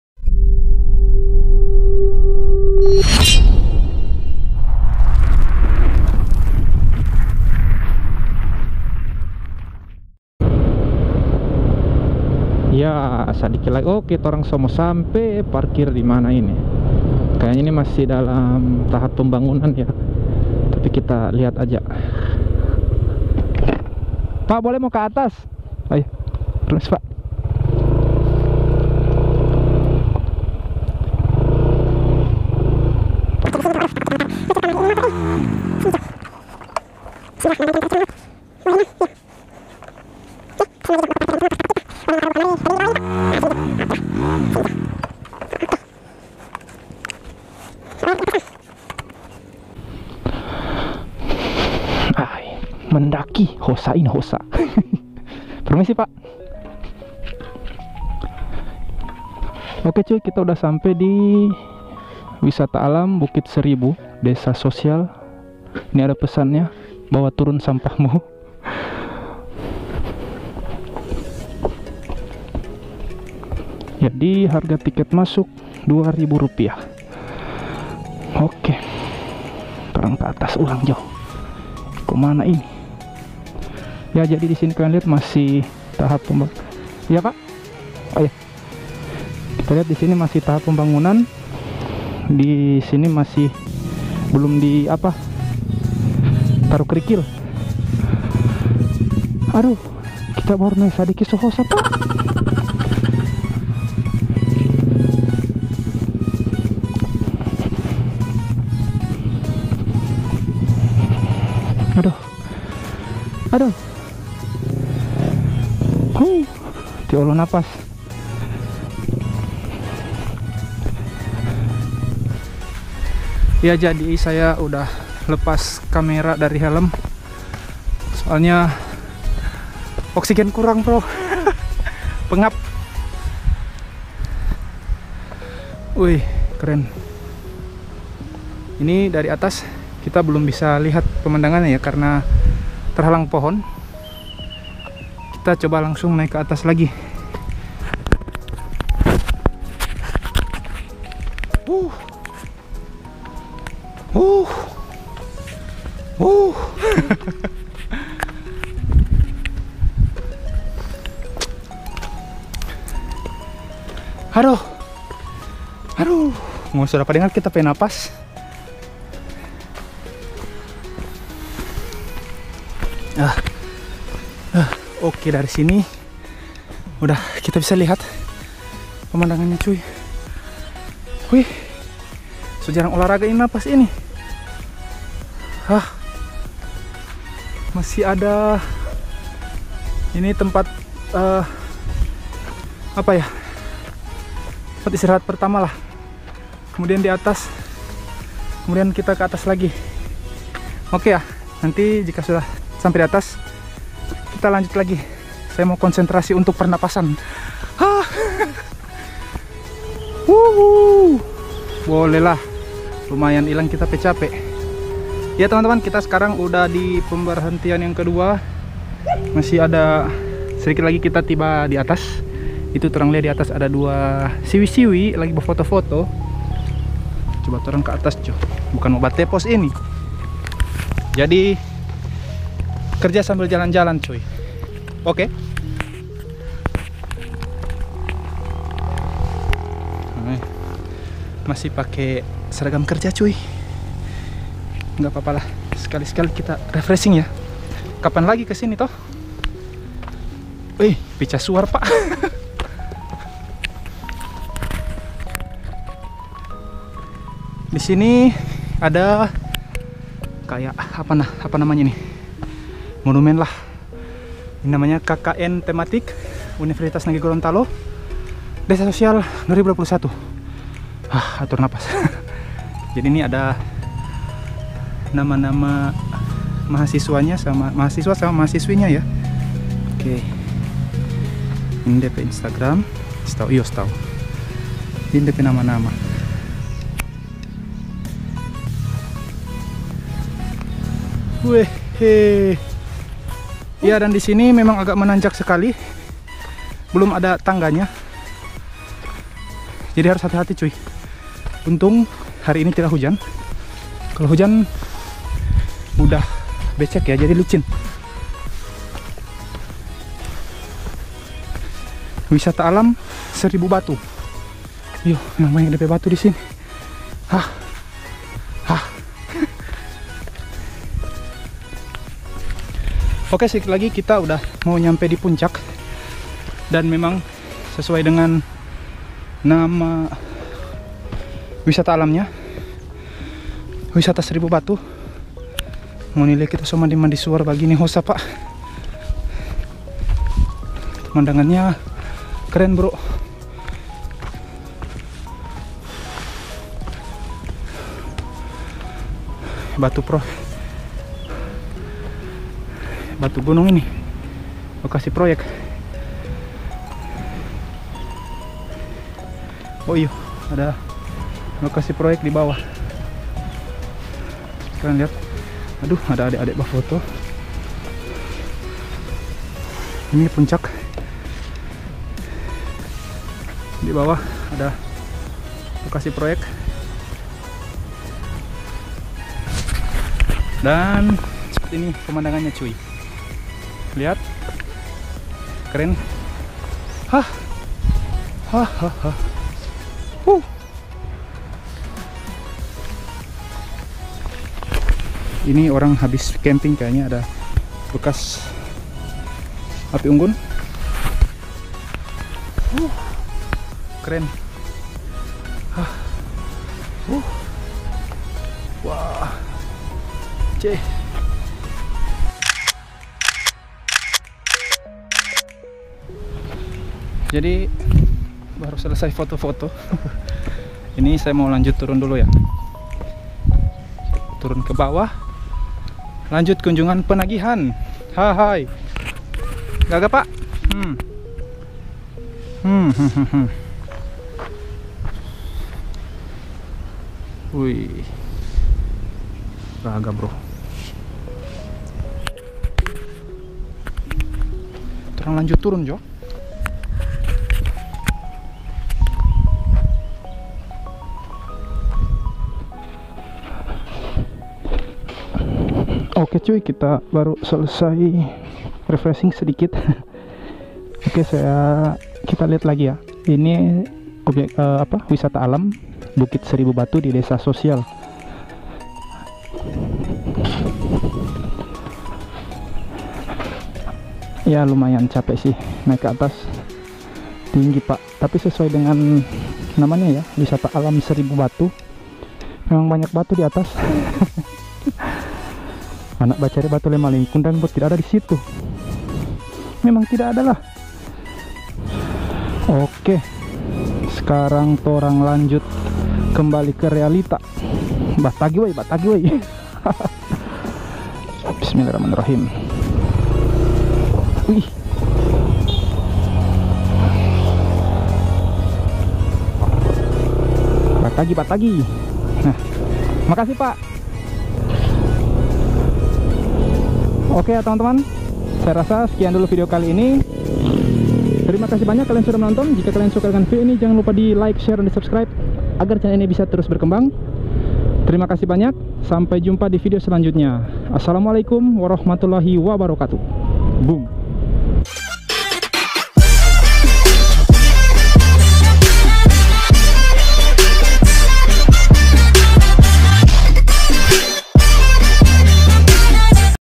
SPOT WISATA BARU DI BOALEMO || BARU NAIK UDAH NGOS NGOSAN
Assalamualaikum..
Setelah kunjungan iseng² mampir di tempat wisata baru,
Terletak di Desa Sosial Paguyaman. Tempatnya masih sementara di percantik lagi oleh warga setempat..

GEAR
Camera    :  Yi Cam versi international Mod
Intercom  :  Vnetphone V6 Mod

TERIMA KASIH